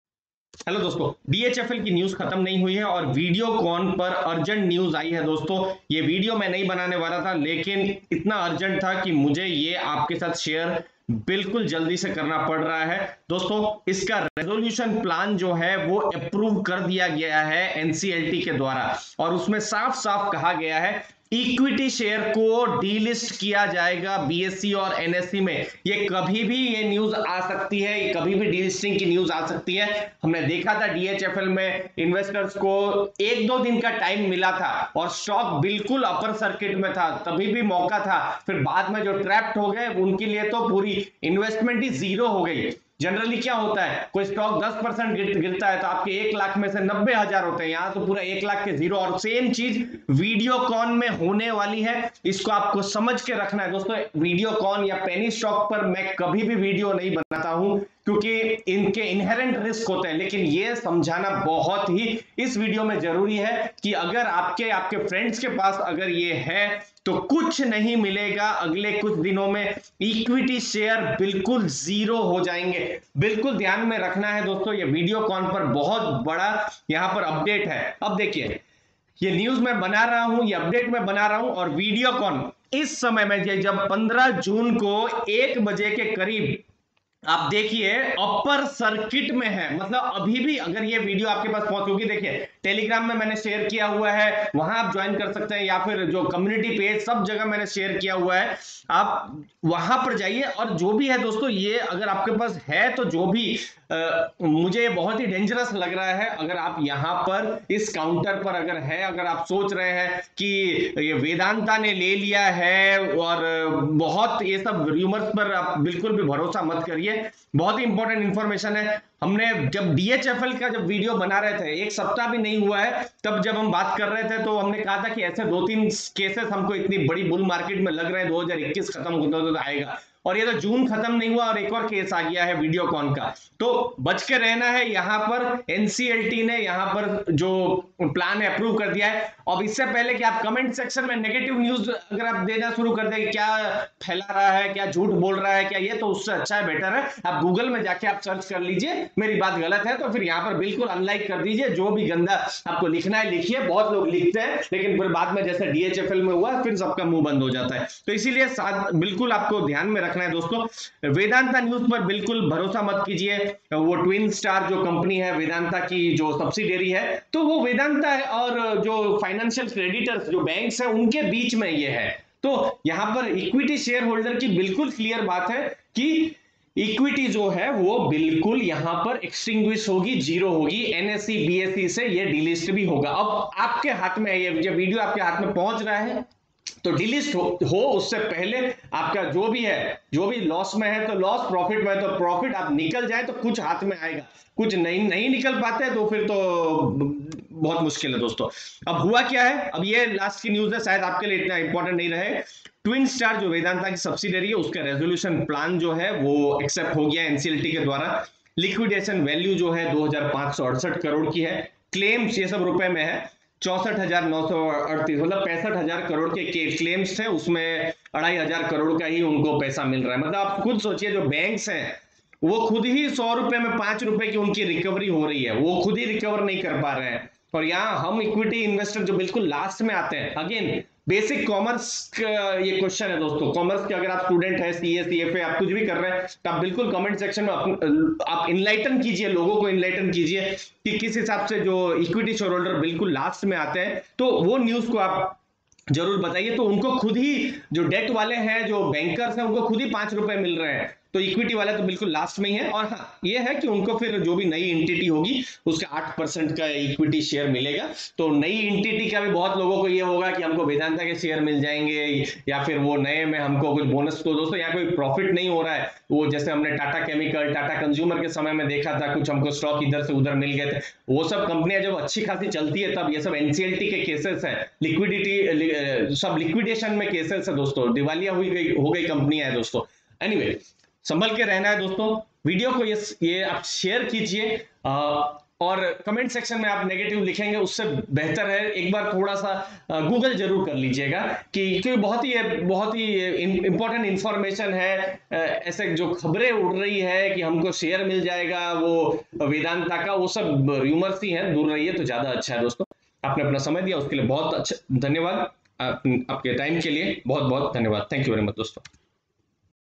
Videocon share delisting | Videocon share trading | Videocon trading suspended
Videocon share will be delisted soon. Videocon share latest news is discussed in the video. videocon trading is suspended.
Our Amazon Storefront for Investment & personal growth books